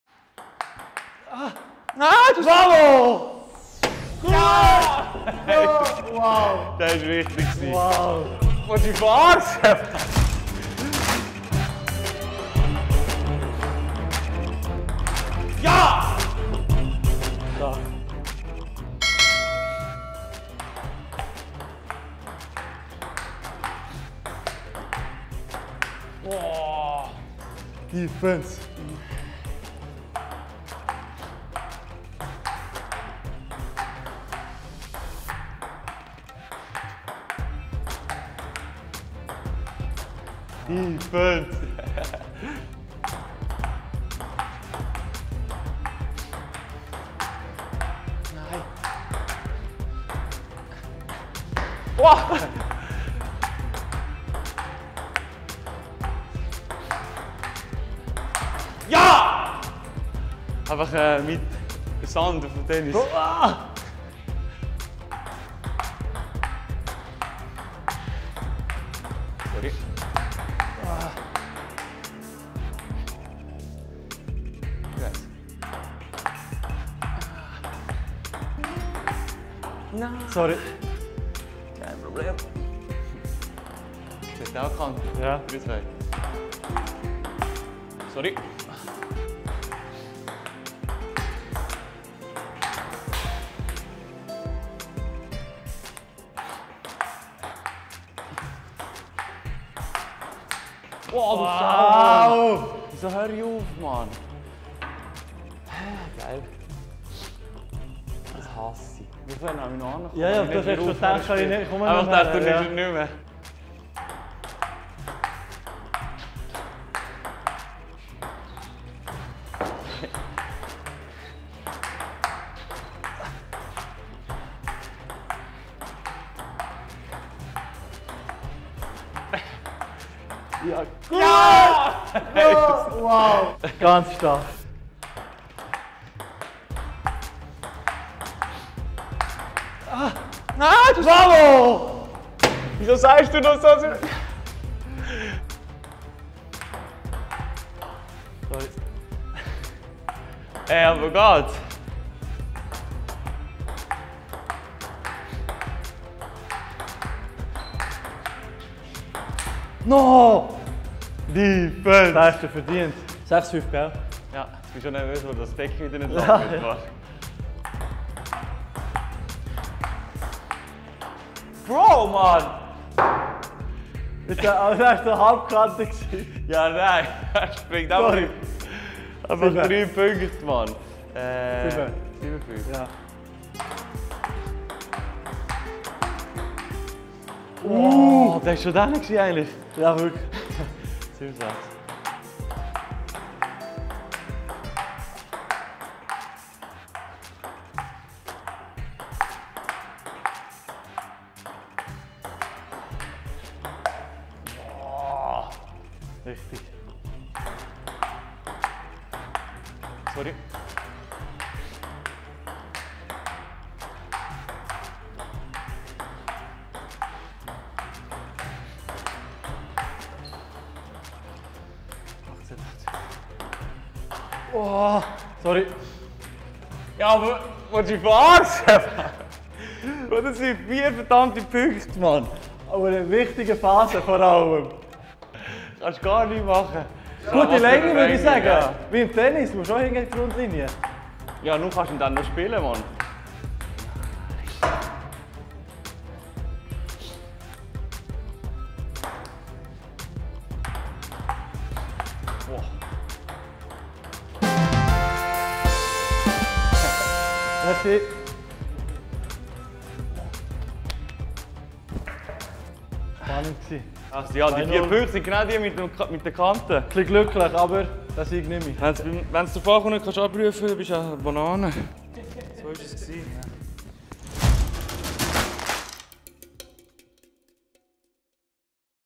Ping-Pong-Duell zwischen Christian Fassnacht und Dario Marzino
In dieser «Skill & Fun Challenge» geht es um Tischtennis. Christian Fassnacht und Dario Marzino wurden von ihren Teamkollegen nominiert zum grossen Ping-Pong-Duell auf dem Tischtennistisch im Meeting-Raum «Geni Meier». Die beiden lieferten sich eine hochstehende und umkämpfte Partie auf elf Punkte.